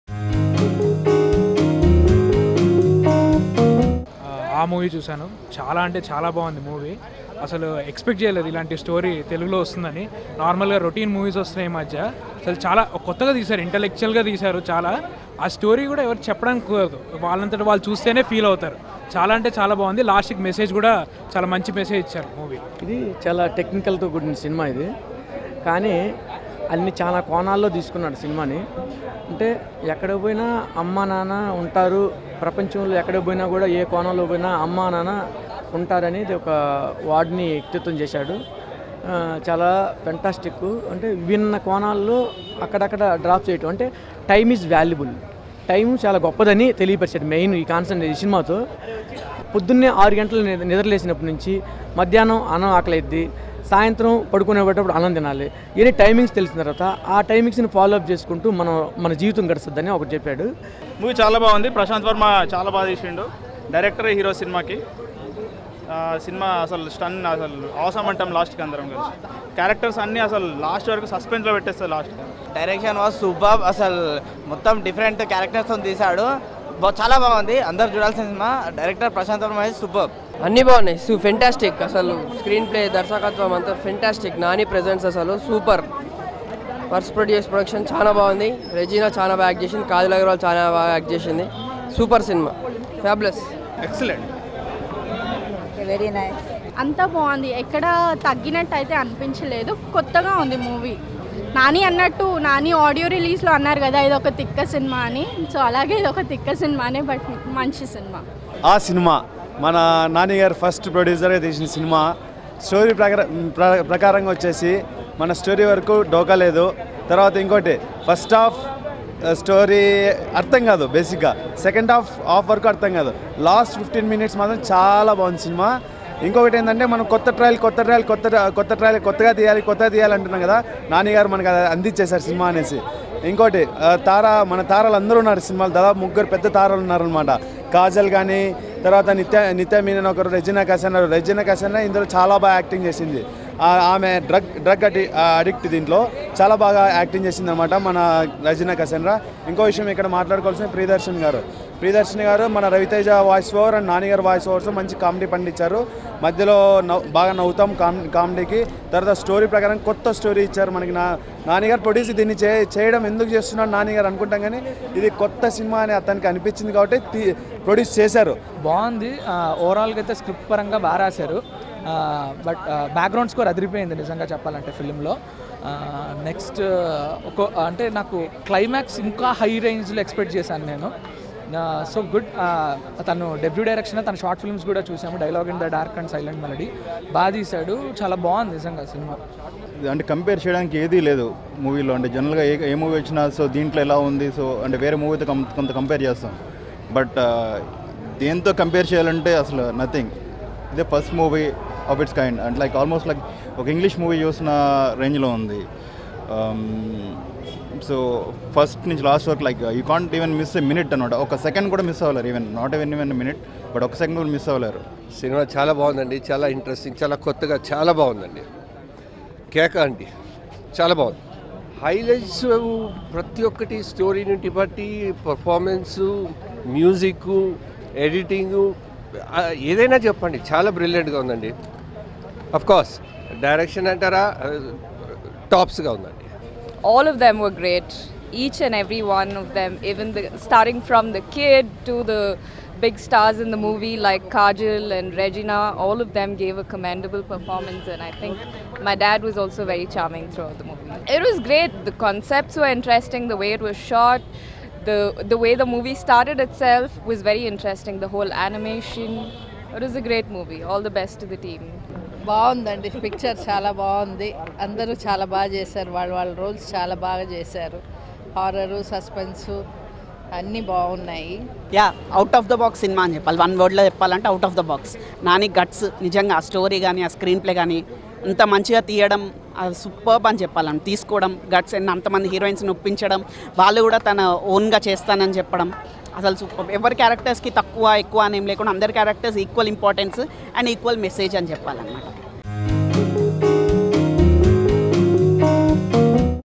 Awe Movie Public Talk | Public Response | Kajal Agarwal | Nithya Menen | Regina | Nani | Ravi Teja
Public gives a positive response to latest flick 'Awe' movie which was directed by the debut director Prashant Varma.